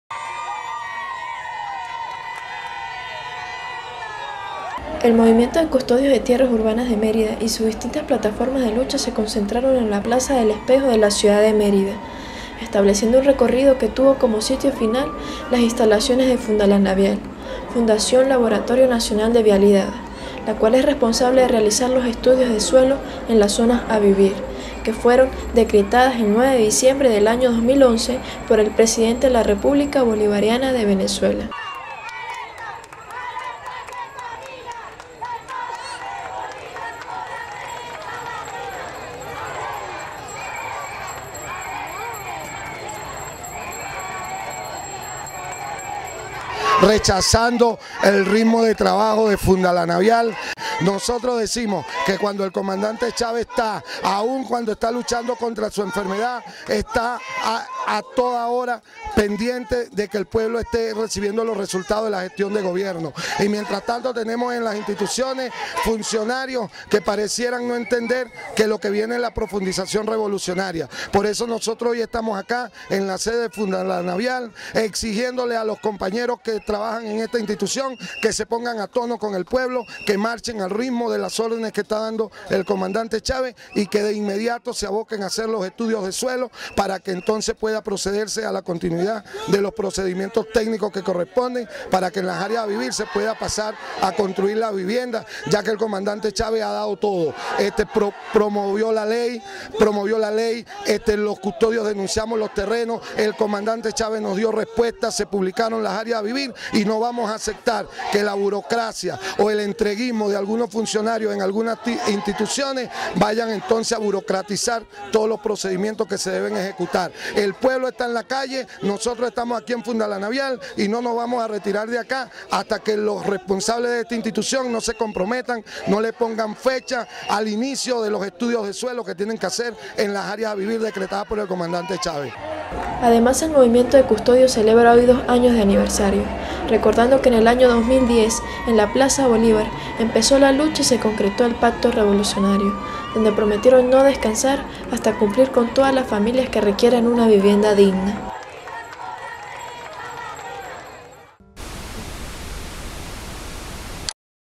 Aqui Tatuy Informa Movilización Custodios de Terrenos Urbanos
El 2 de marzo el Movimiento de Custodios de Tierras Urbanas de Mérida y sus distintas plataformas de lucha se concentraron en la Plaza del Espejo de la ciudad de Mérida. Estableciendo un recorrido que tuvo como sitio final las instalaciones de FUNDALANAVIAL (Fundación Laboratorio Nacional de Vialidad), la cual es responsable de realizar los estudios de suelos en las zonas AVIVIR, que fueron decretadas el 9 de diciembre del AÑO 2011 por el presidente de la República Bolivariana de Venezuela .

Esta actividad tuvo como principal finalidad expresar el rechazo al ritmo de trabajo que lleva a cabo la institución, y del mismo modo exigir a los representantes que se comprometan con los inicios de dichos estudios, para poder seguir con el procedimiento que requiere la creación de las viviendas.

Además el movimiento de custodios celebra hoy 2años de aniversario, recordando que en el año 2010 en la plaza Bolívar, empezó la lucha y se concreto el pacto revolucionario, donde prometieron no descansar hasta cumplir con todas las familias que requieren una vivienda digna.
Los custodios de tierra reclaman, de forma organizada por el apoyo gubernamental y seguirán movilizados hasta lograr sus objetivos